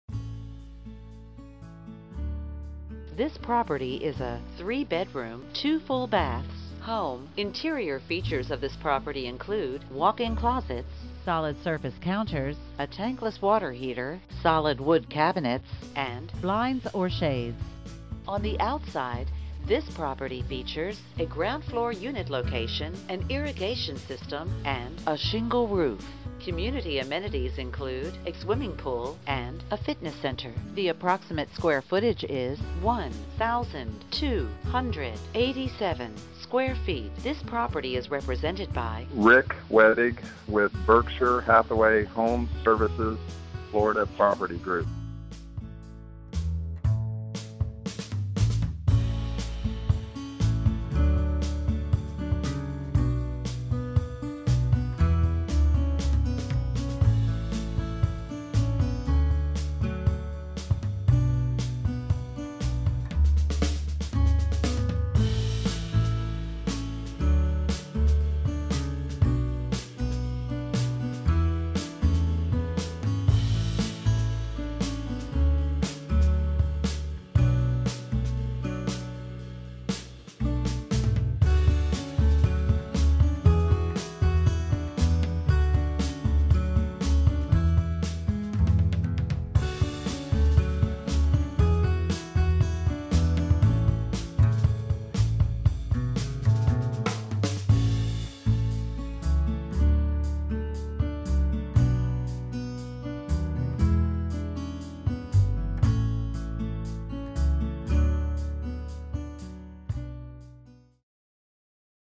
2717 N POINCIANA BLVD #177, Unit 177, KISSIMMEE, Fl, 34746 Tour - $129,900
Represented by Richard Wedig
New Tampa Office
3 Bedrooms - 2 Full Baths
Immaculate 3/2 deluxe villa, renovated from top to bottom. Ground floor, European-style flat with granite countertops, raindown stone showers, ceramic tile flooring, new energy efficient windows, 14 SEER rated HVAC (2015), tankless water heater (2016), updated closets, with elegant fixtures throughout. Accessible from all major roads (I-4, 417, 535, 192), and minutes from Disney, Universal Studios, Sea World, Multiple Golf Courses, Premium Shopping, Dining, and Entertainment. Ideal location for investors, zoned for short- or long-term rental. This unit is truly one of a kind. As appealing as the pictures may look, they do not do it justice. Come see how attainable local luxury can be.